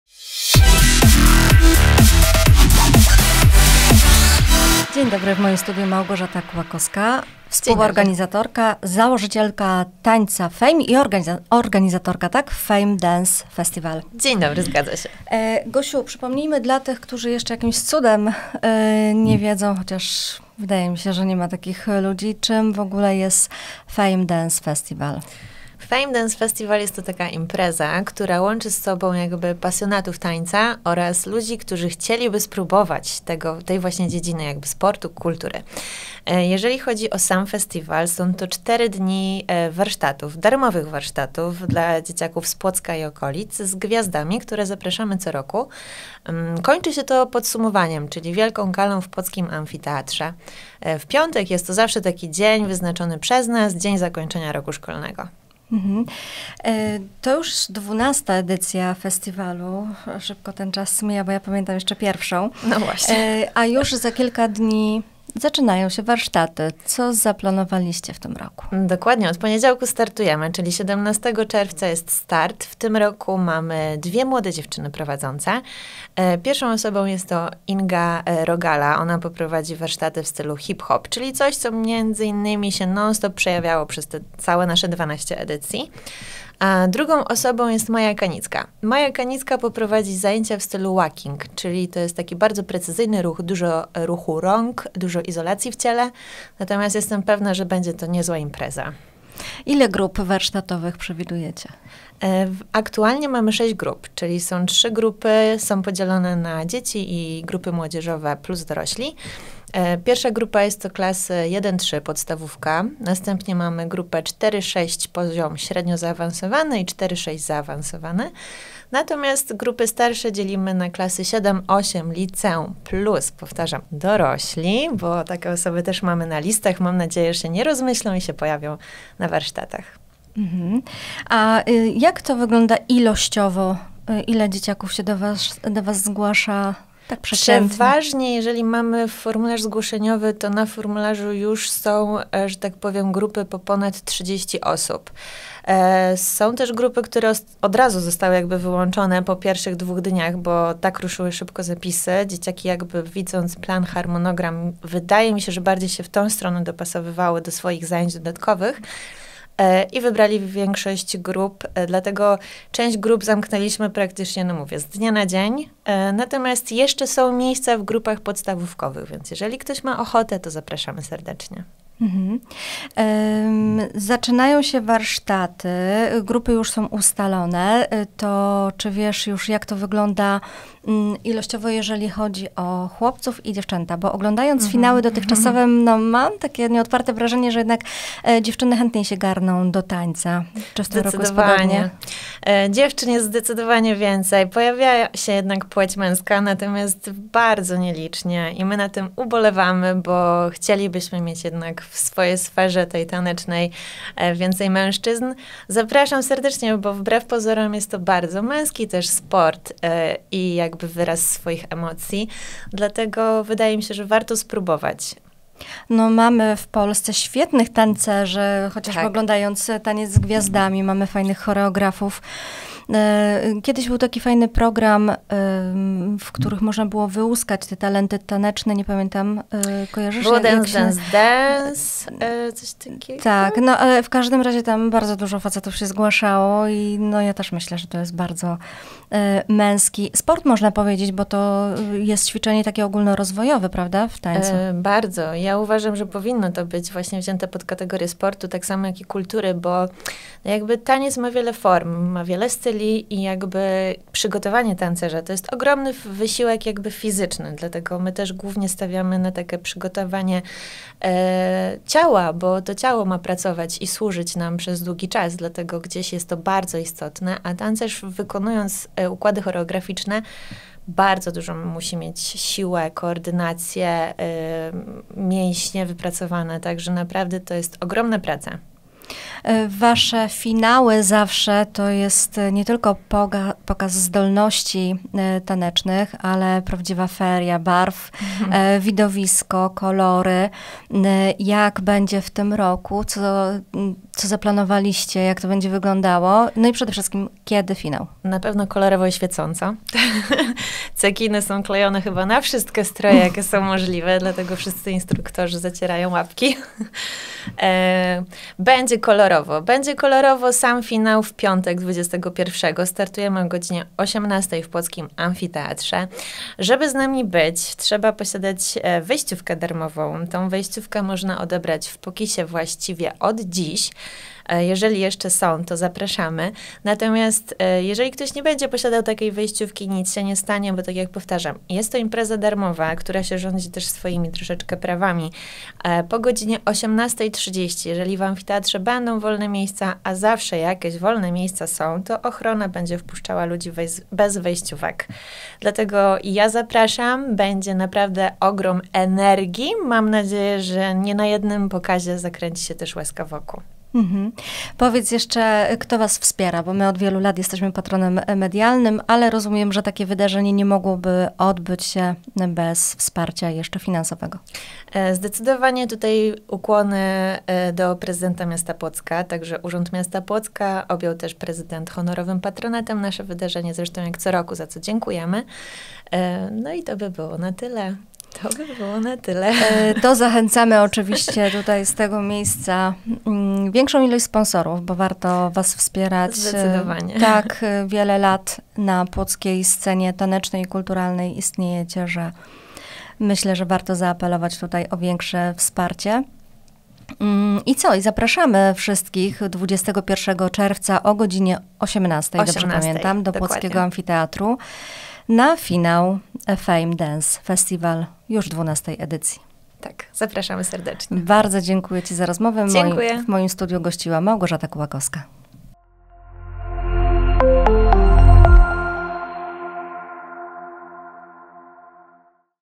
12. Fame Dance Festival w Płocku
Co zaplanowano podczas 12. edycji Fame Dance Festival? Kto tym razem będzie trenerem grup warsztatowych? I kiedy odbędzie się finał? O tym rozmawiamy z Małgorzatą Kułakowską, organizatorką Fame Dance Festival.